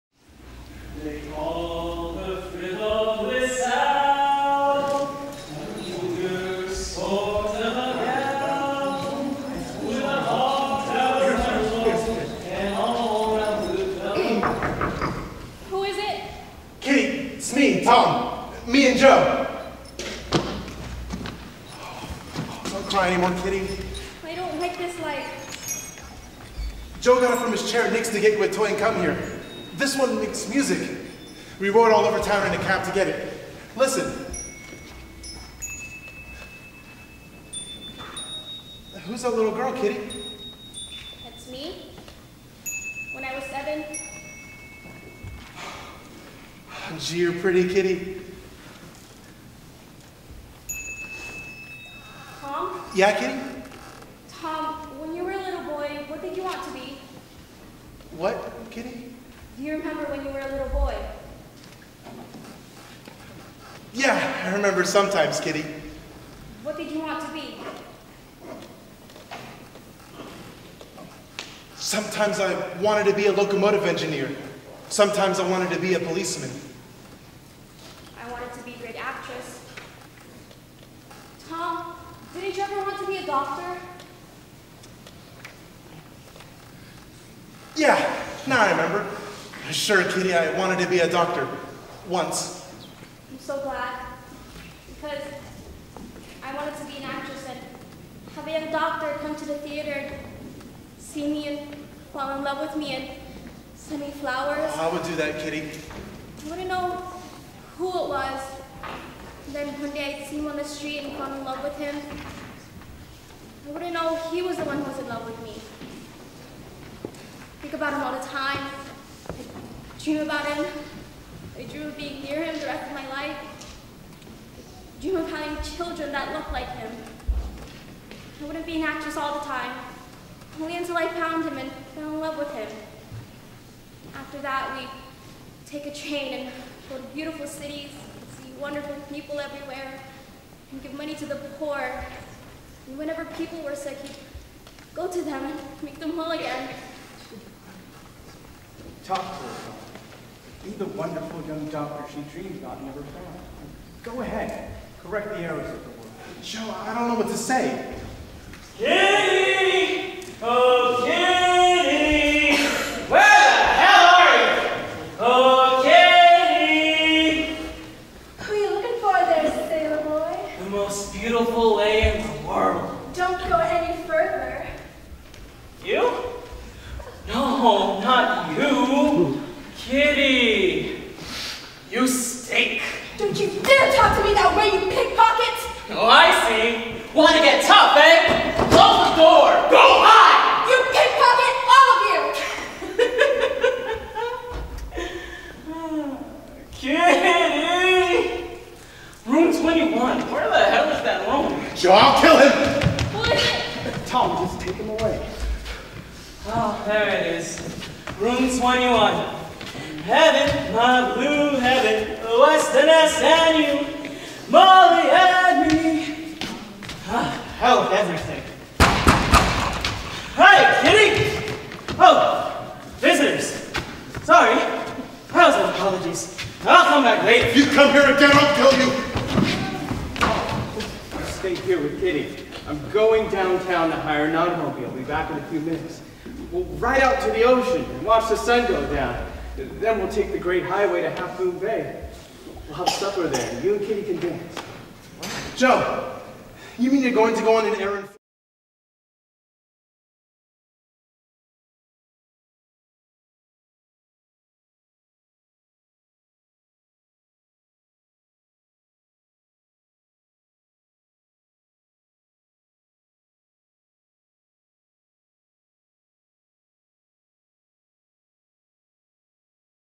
PAS 2015 "Excerpt from the Time of Your Life, Pt. II" Drama Club
Act 3, Scene 1 from “The Time of Your Life” (William Saroyan) 1940 Pulitzer Prize

SETTING: Nick's Pacific Street Saloon, Restaurant and Entertainment Palace, San Francisco, 1939

TOM: Lisa Olson; JOE: Steven Marshall; KITTY DUVAL: Rorie Azucena; KILLER: Vanessa Fitzpatrick; THE SAILOR: Deo Gammad